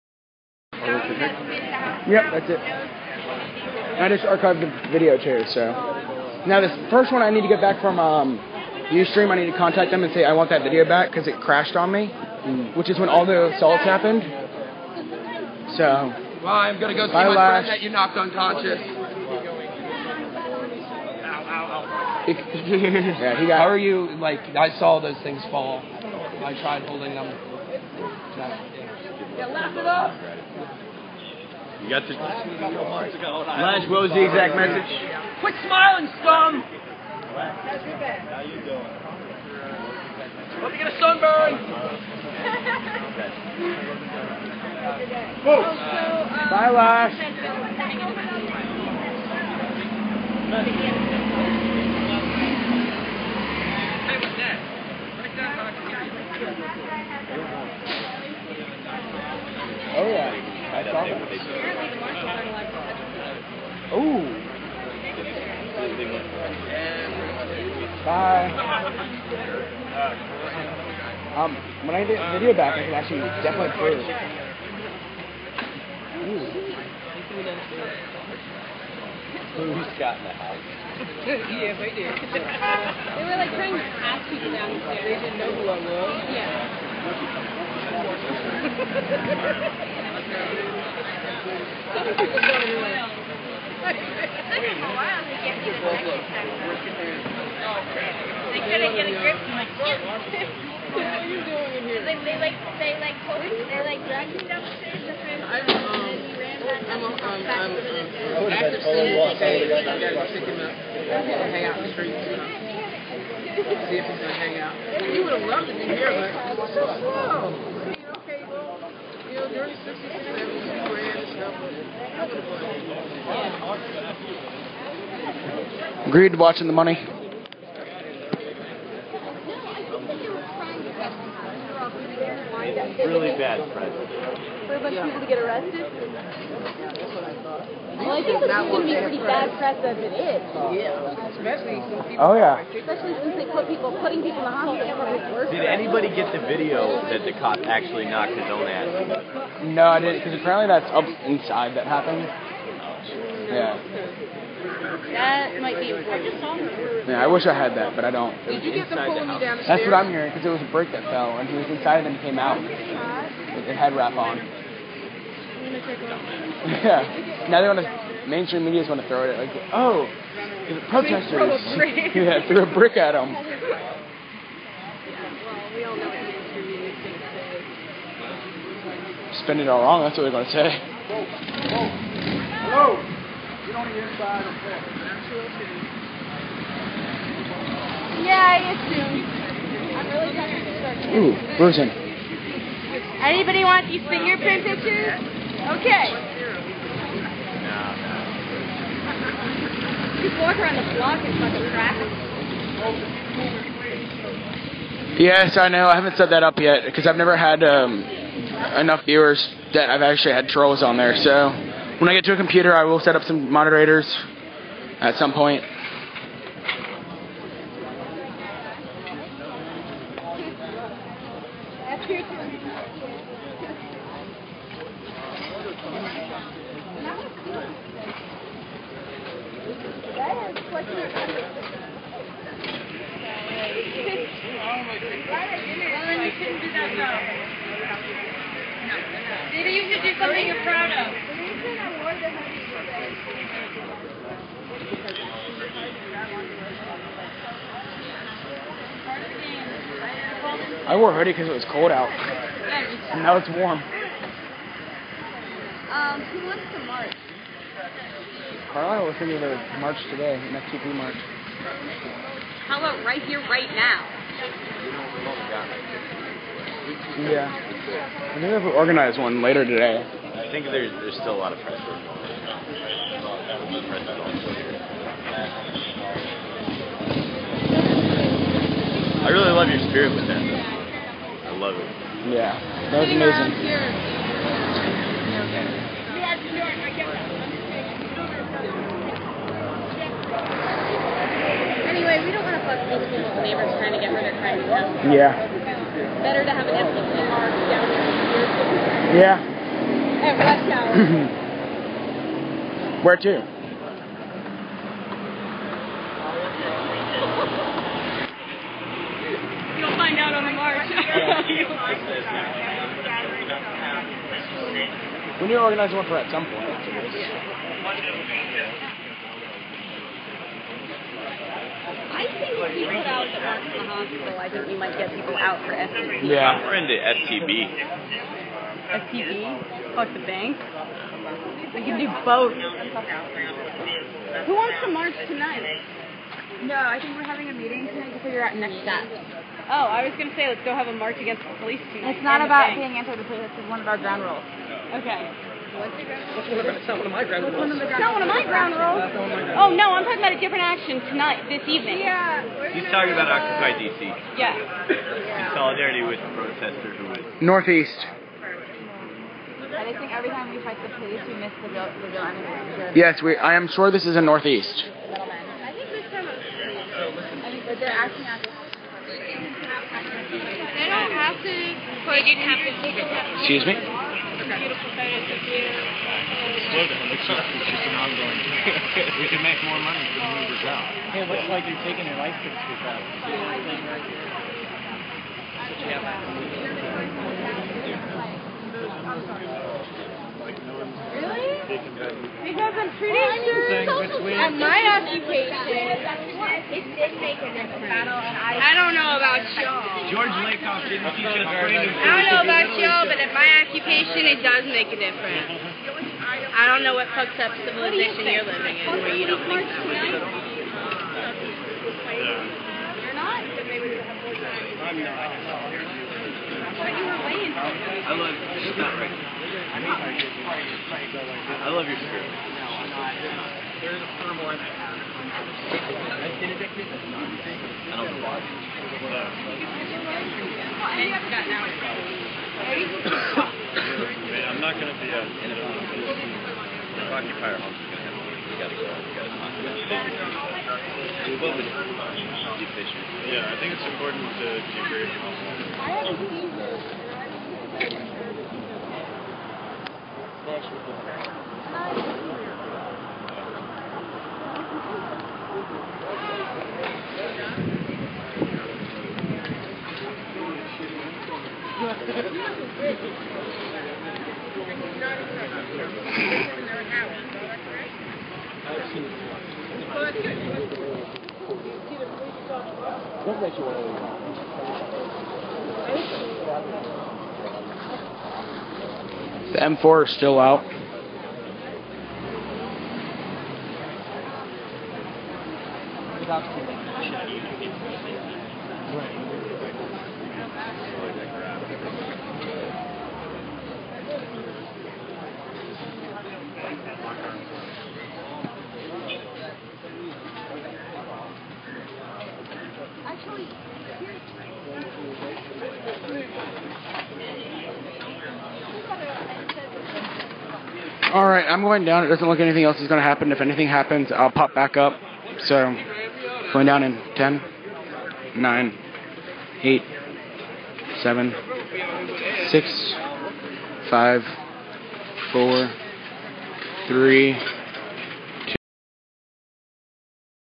June 5th eviction cleaning out the house
on june 5th after the us marshals put someone in the hospital they got the moving crew to come clear out the house of all belongings. this si just come of the footage from them clearing out the house.

please help me continue live streaming by donating to my wepay account. the link to donations is